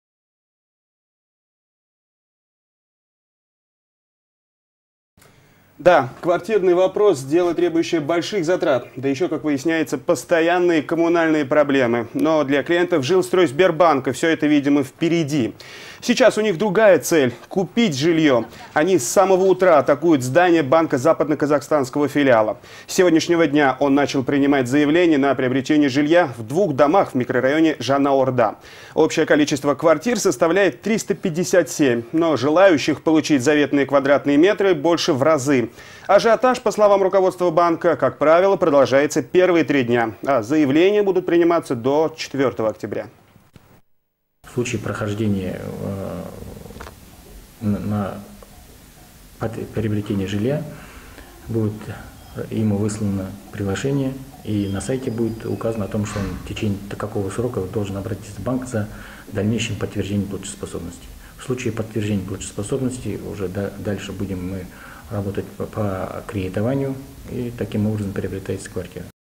Квартирный вопрос
С самого утра клиенты Жилстройсбербанка атакуют здание Западно-Казахстанского филиала.